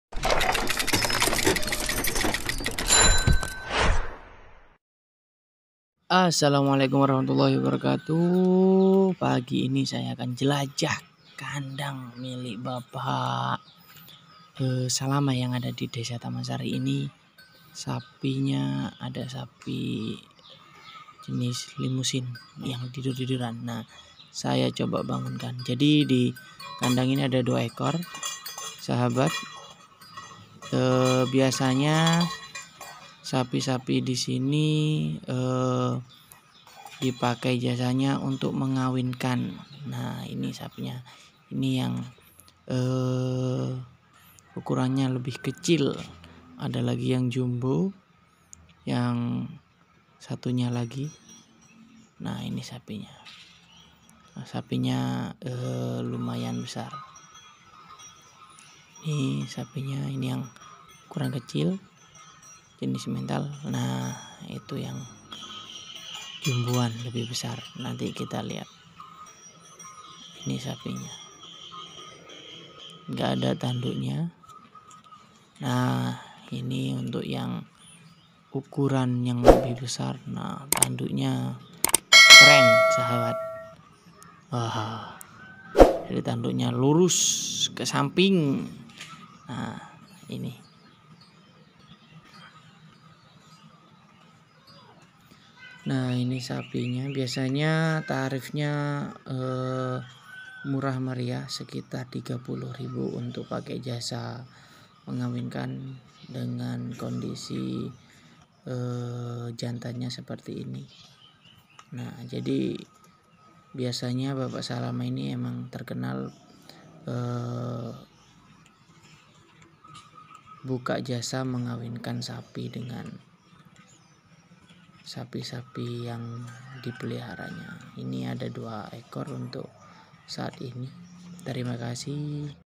HANYA 30K JASA MENGAWINKAN SAPI
jasa Kawin di kandang milik pak. Salamah di desa tamansari, biasanya para warga membayar 30 ribu saja dengan pejantan lumayan jumbo dan ganteng